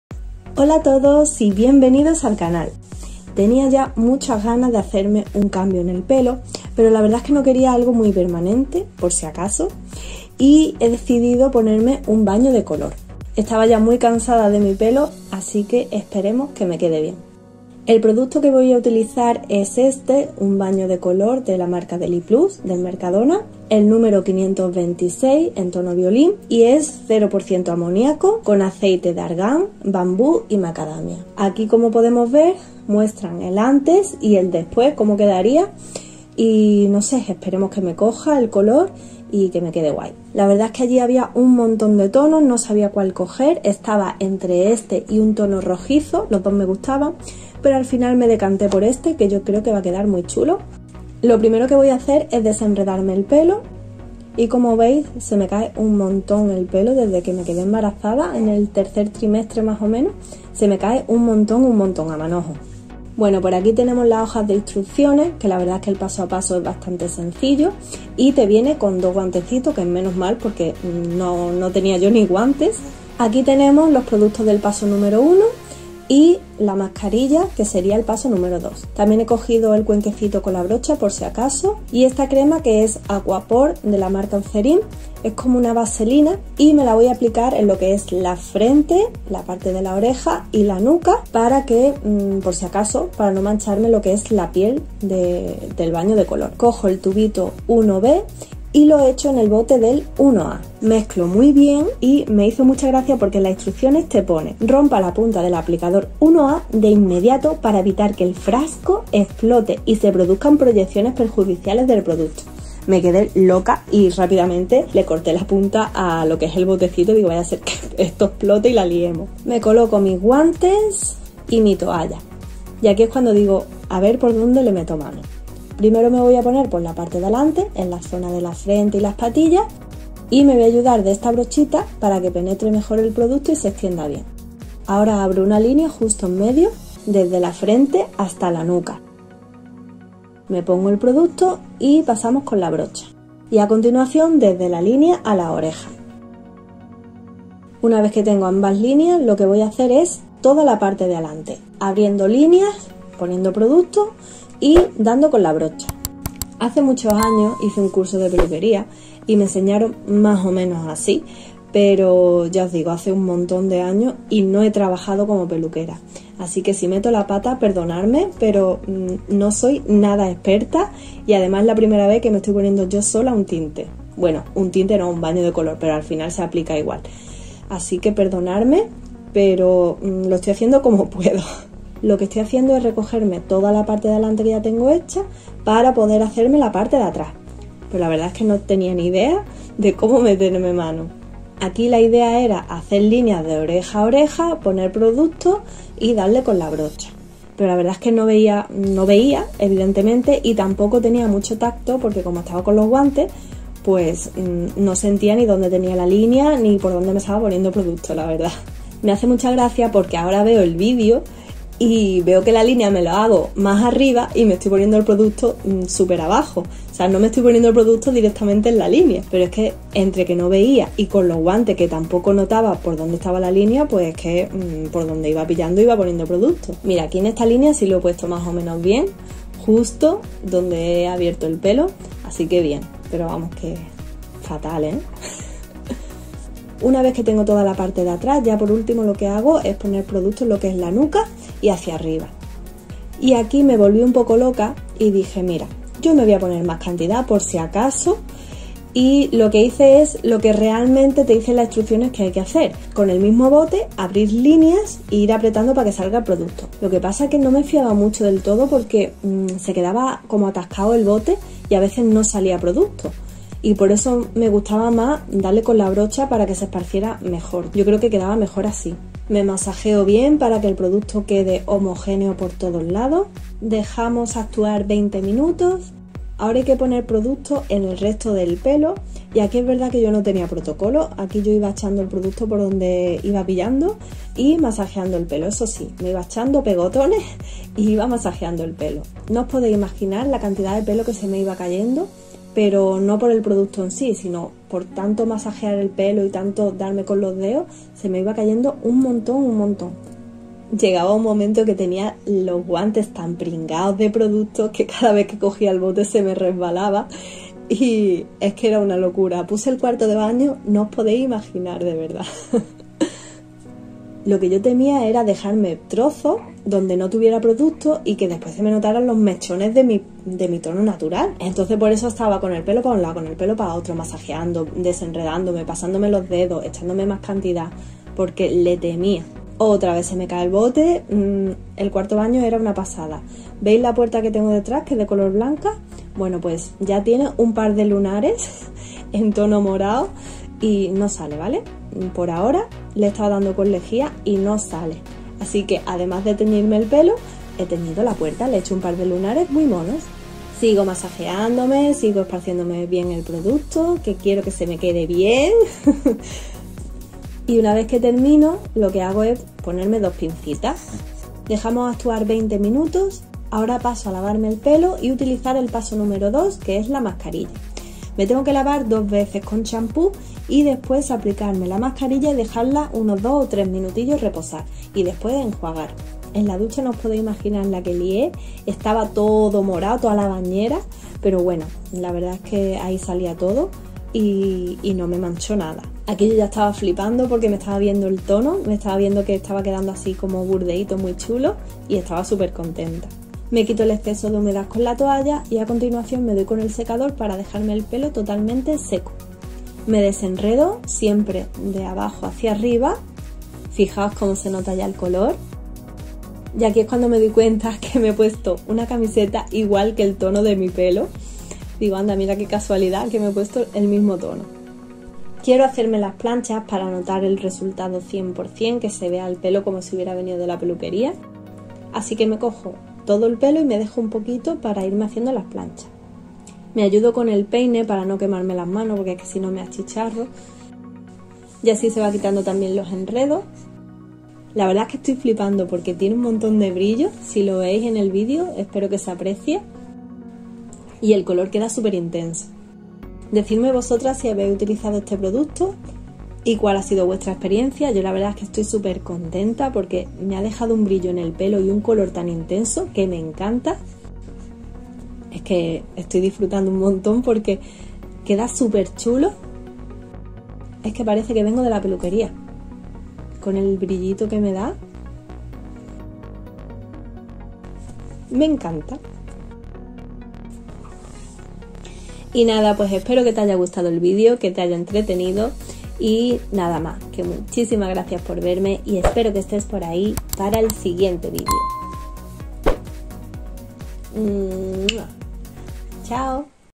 🔥¿Cómo APLICAR BAÑO de COLOR SOLA en CASA?🔥(Paso a paso) DELIPLUS
Que ganas tenia de hacerme un cambio en el pelo... pero no queria algo radical, asi que he decidido ponerme un baño de color de la marca DELIPLUS de MERCADONA.
Es el numero 526 en tono VIOLIN.
¿QUE TE PARECE EL CAMBIO?

UN BESO MUY GRANDE
MUUUUAKS!!!